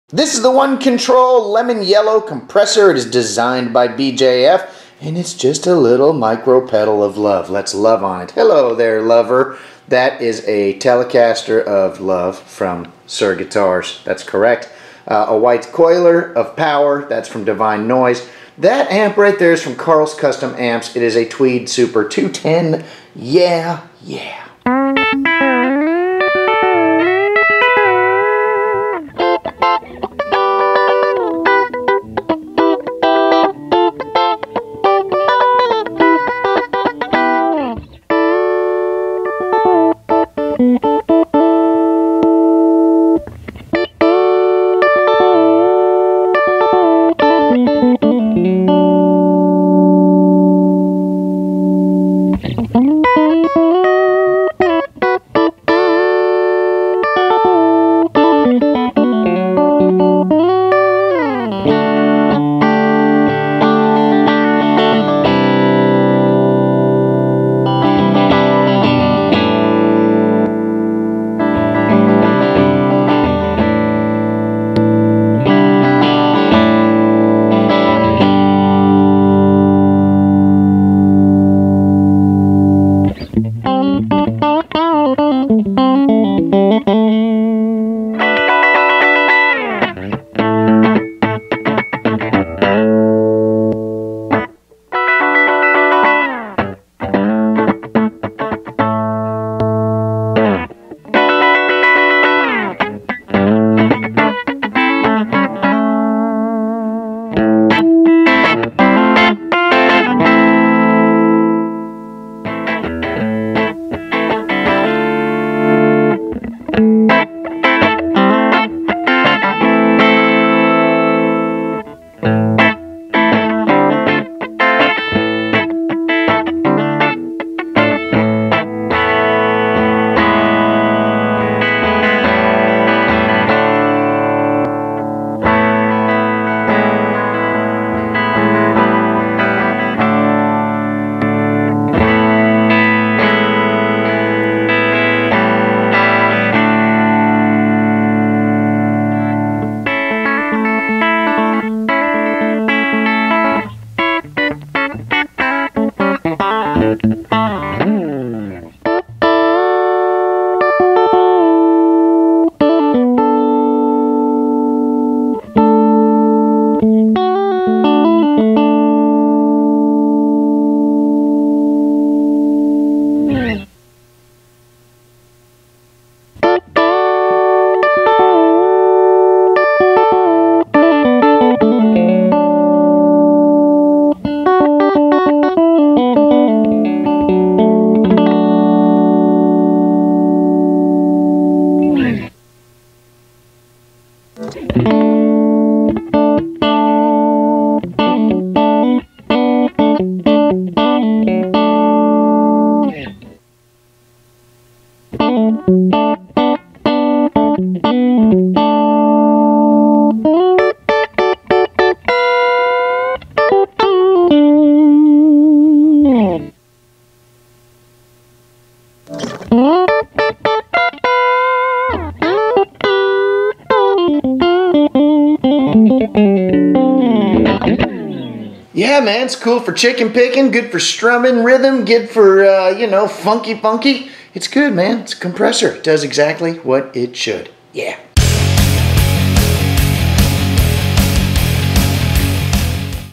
One Control LEMON YELLOW COMPRESSOR mini pedal demo with Suhr Tele
Groovy little micro compression pedal of love from Onecontrol.

I used my Suhr custom fairy dust Telecaster & Carls Custom Amps TWEED SUPER 210 combo. Divine Noise white straight and coily cables of love...mic'd up TELEFUNKEN M-80 and M-81 dynamic Microphones of love. YEP!